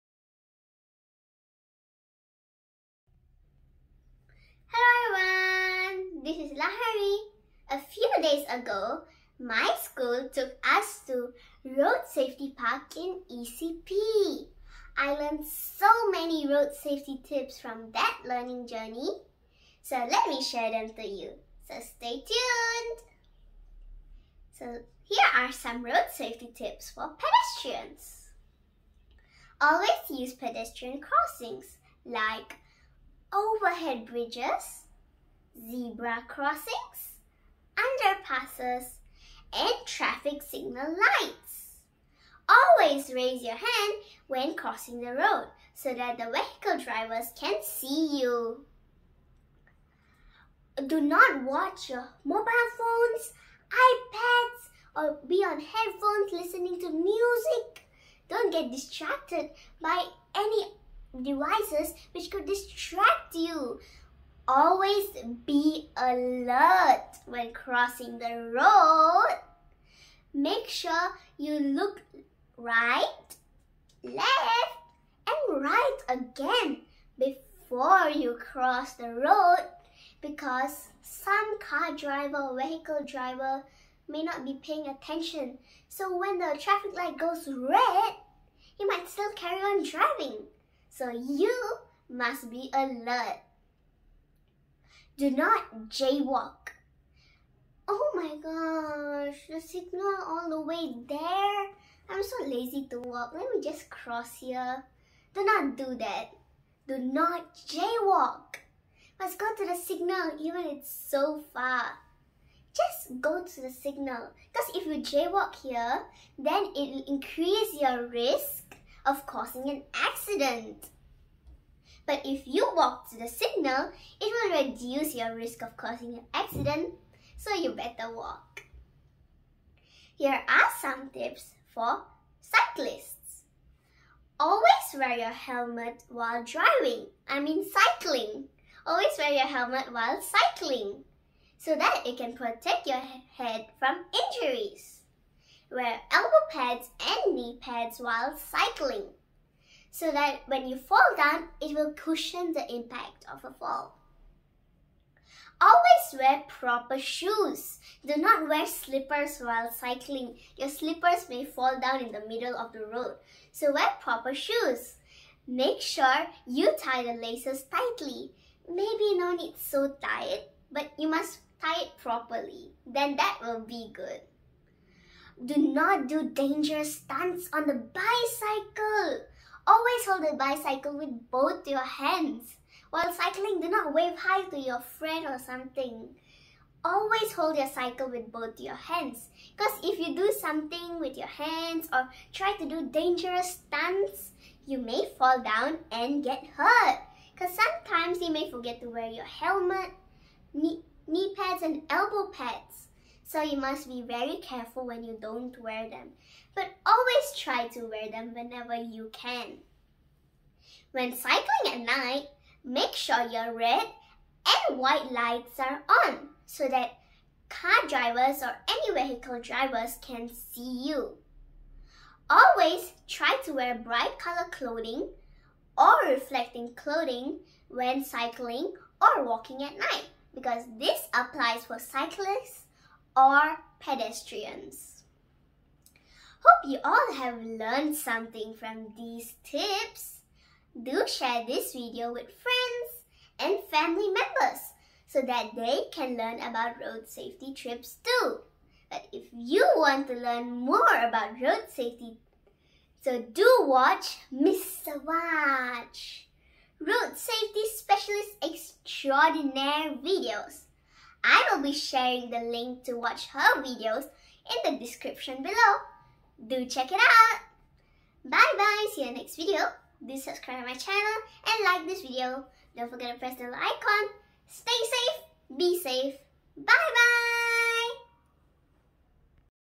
Road Safety Tips for Pedestrians and Cyclists
We can all play a part in keeping our roads safe; all it takes is a little road sense and following a few road safety tips. Traffic accidents can happen in a split second, leaving road users with very little time to react. They need to be alert all the time. Avoid distractions all the time to prevent injuries to oneself and others.

In this video, let's see a few tips for Pedestrians and cyclists.

You could find more tips from videos below. Watch these, enjoy and follow the safety tips to be safe :)